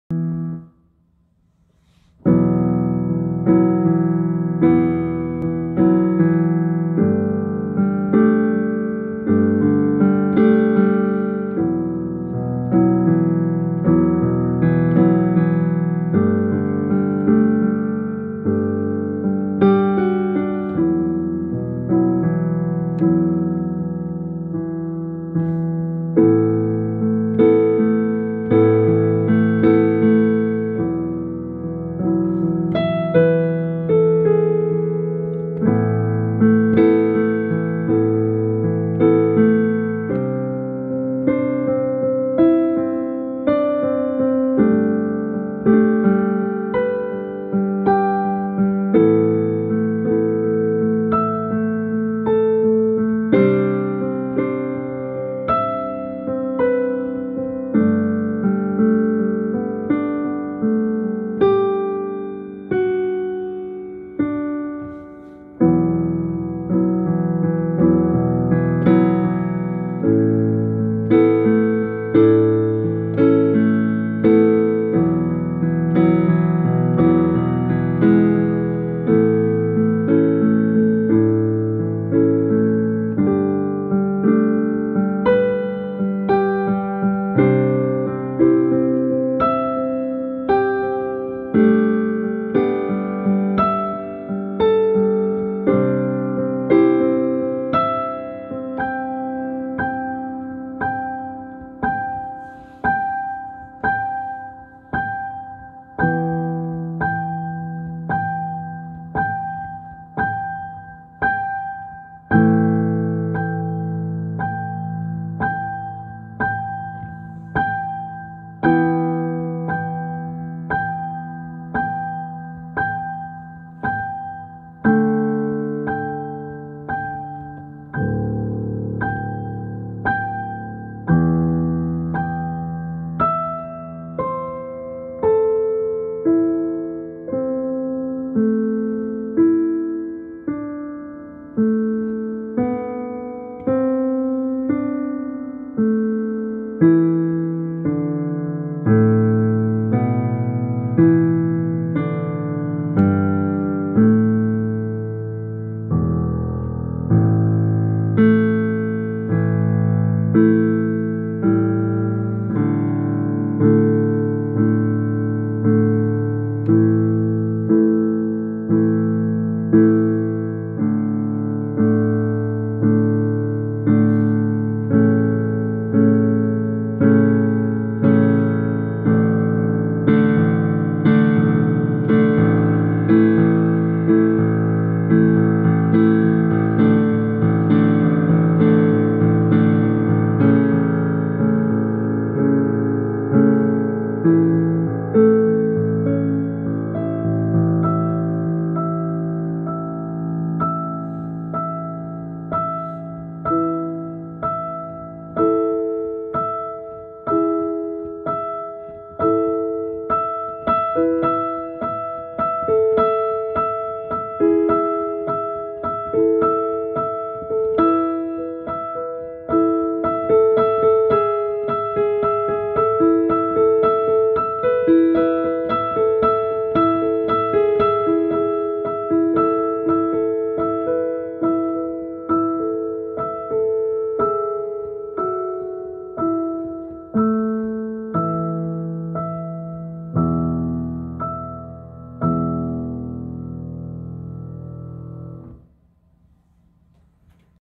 nottooperfect 08
Random piano playing. Minimal rehersal, mostly played on the fly.